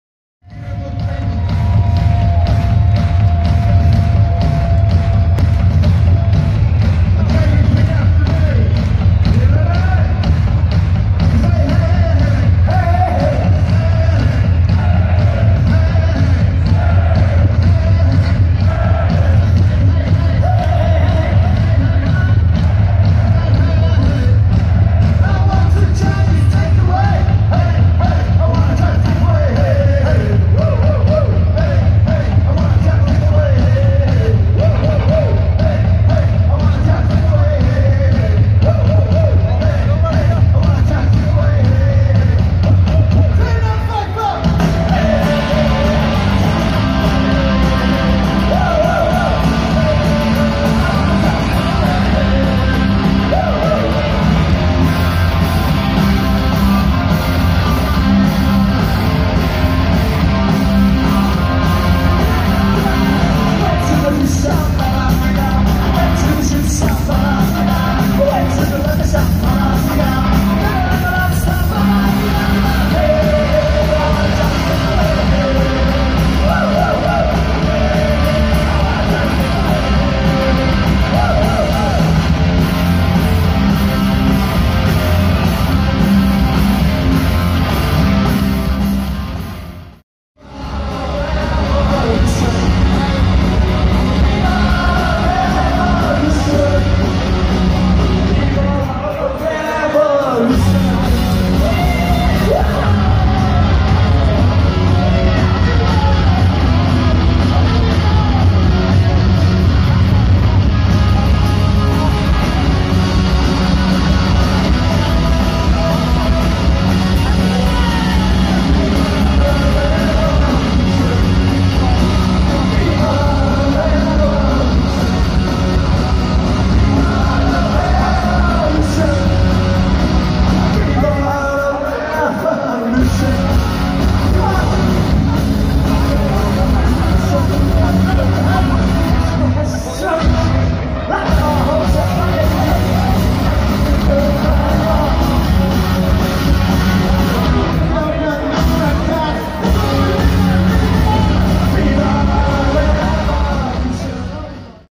The Adicts - (Medley)@Arena Leipzig 2017
Vorband The Adicts (Medley Live)am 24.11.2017 die Toten Hosen Laune der Natur Tour in der Arena Leipzig